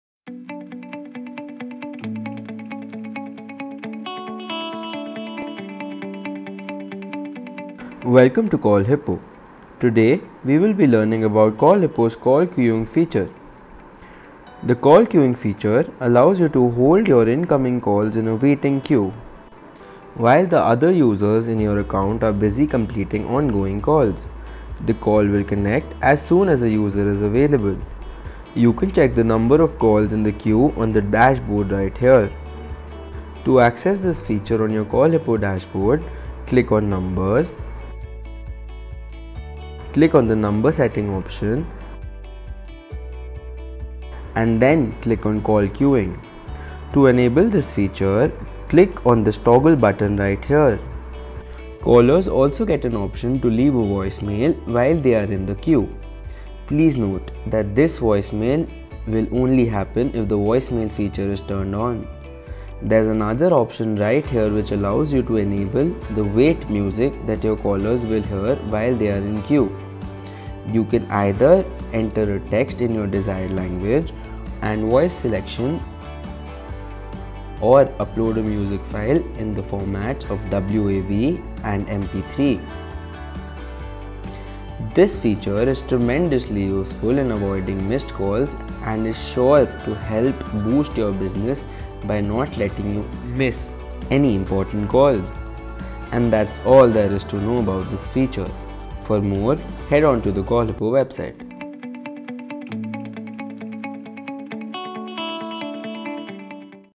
Call Queuing: Call Center Queuing Feature for virtual phone number | CallHippo Feature Video
How to enable call queuing? CallHippo virtual phone solution lets you handle incoming calls and places them in line to be answered when your team is busy with other calls.